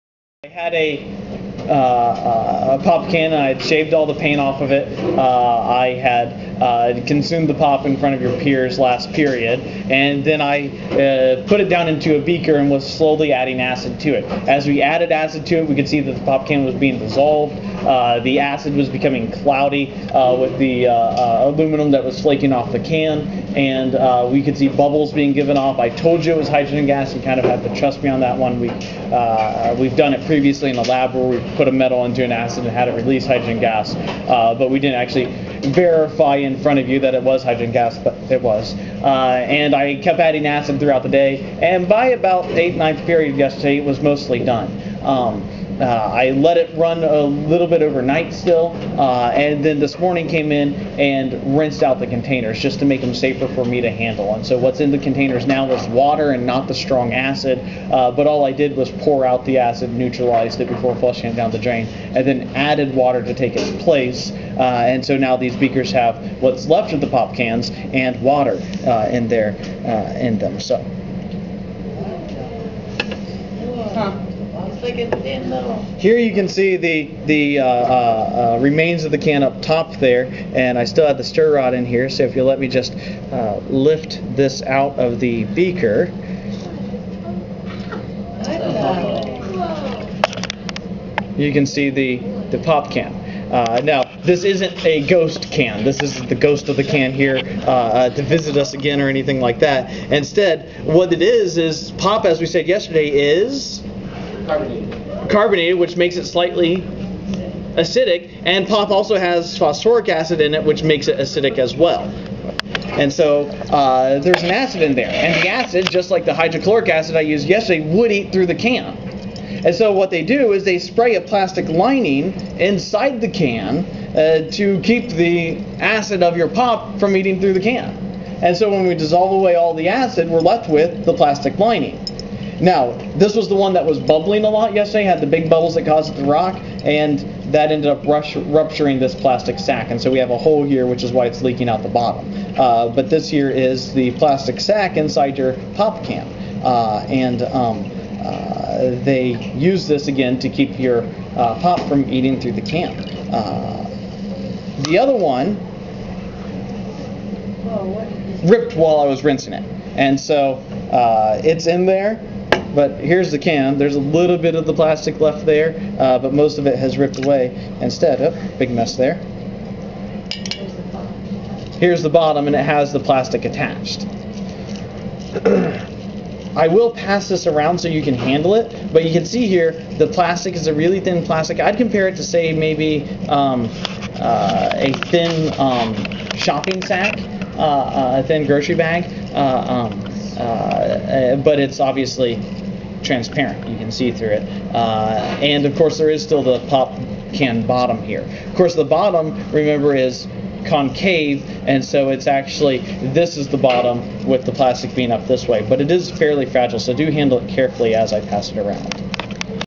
Dissolved Pop Can 02 0809
Here I show the results of a chemistry demonstration I had started in class the day before.  A pop can had its paint sanded off, was emptied, refilled with water, and dangled in a beaker.  The beaker was then slowly filled with hydrochloric acid to dissolve away the aluminum can.  The plastic liner from inside the can is left behind.  [For the first part of the video I am reviewing what happened in class yesterday and the camera is looking at the spot where I will put the "ghost can".